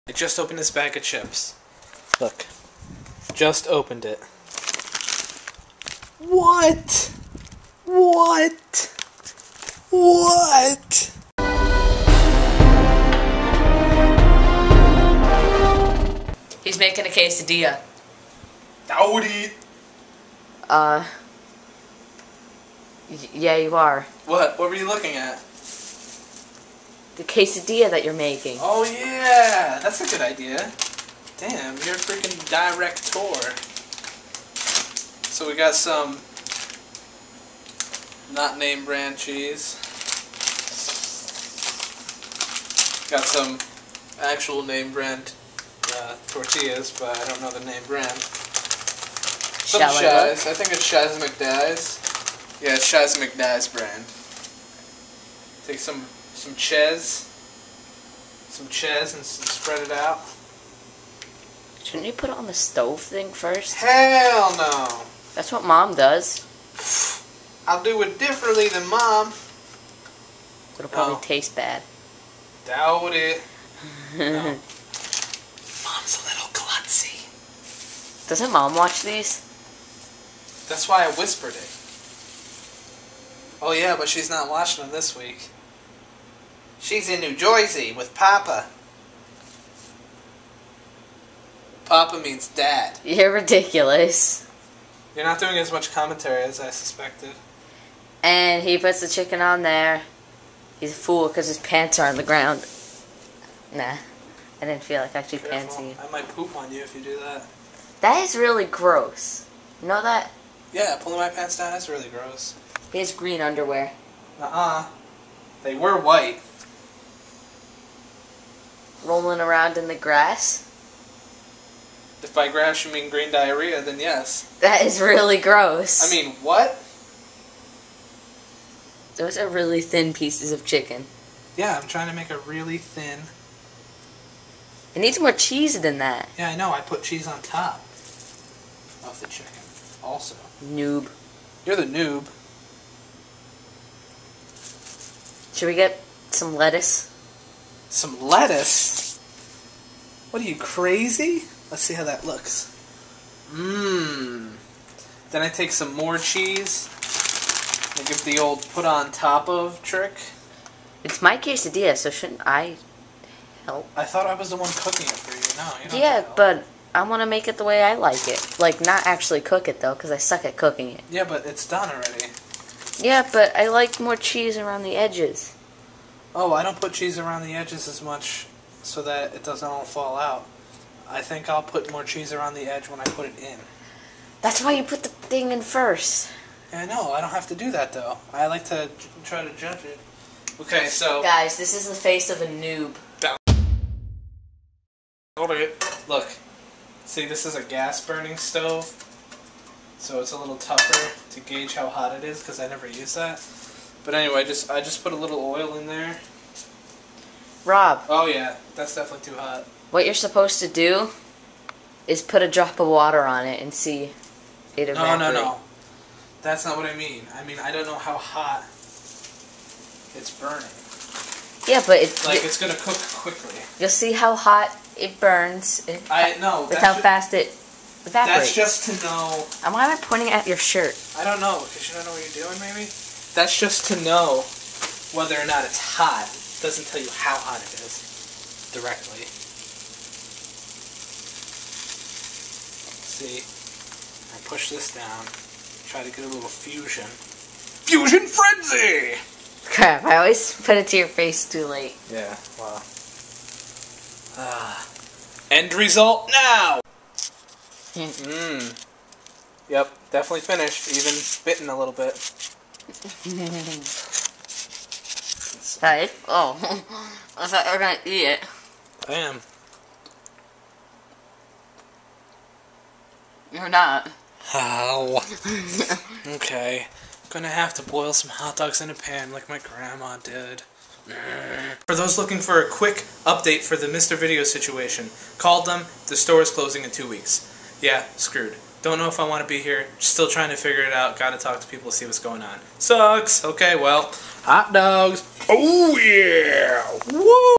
Sheiz McDeiz Brand (DailyVary #63 - 7/20/2010)
Some child prodigy films some fat dude n00b making a quesadilla and then the fat dude complains about stuff, like usual.